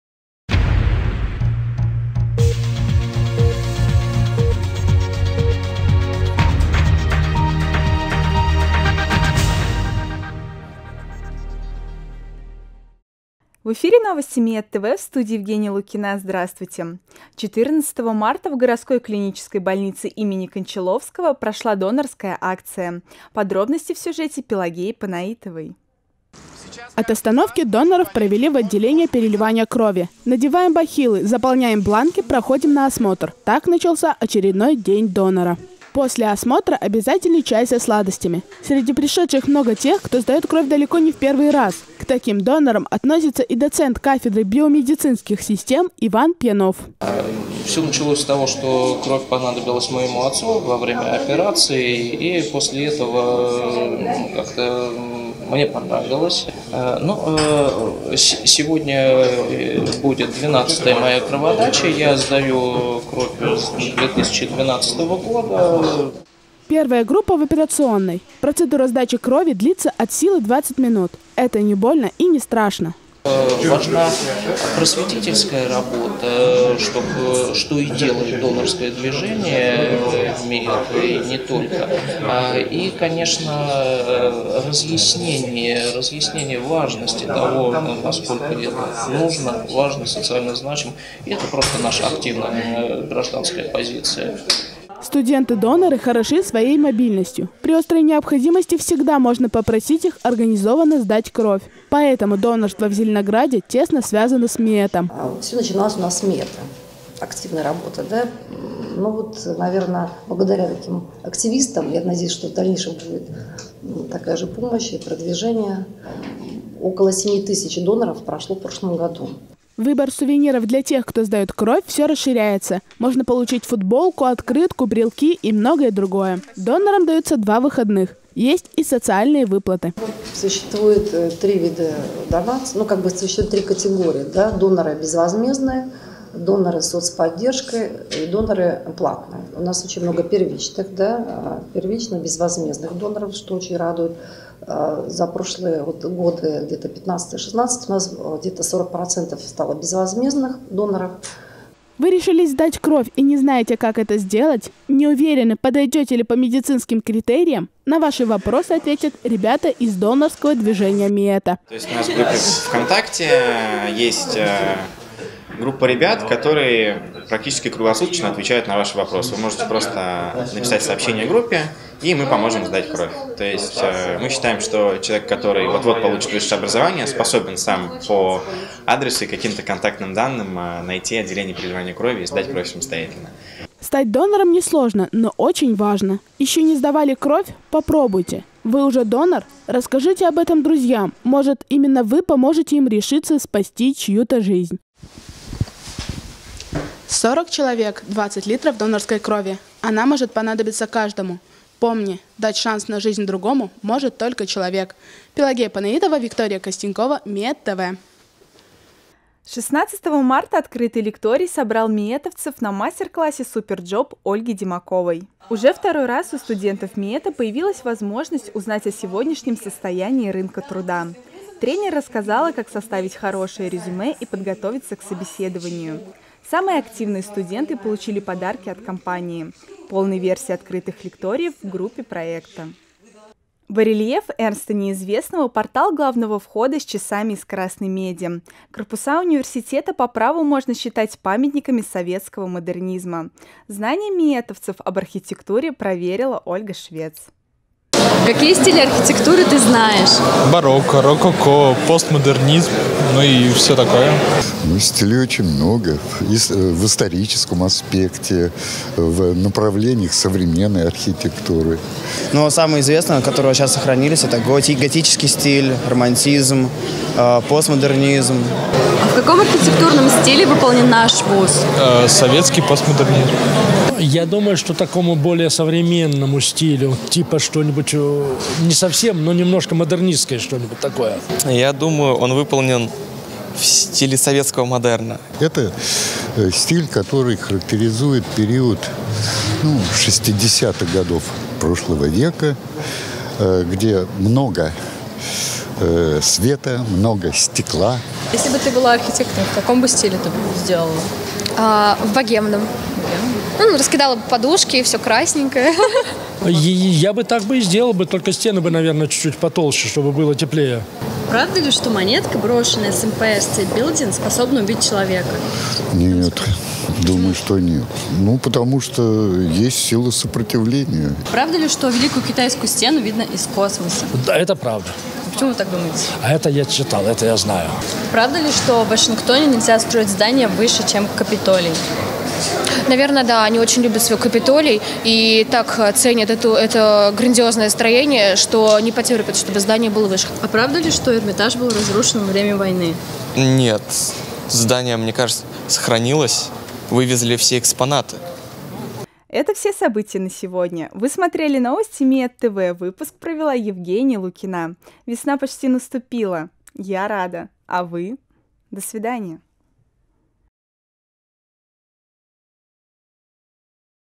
Новости 2017-03-22
Выпуск новостей МИЭТ-ТВ от 22 марта 2017:
— День донора
— Открытый лекторий: МК тренера компании SuperJob
— Опрос об архитектуре МИЭТа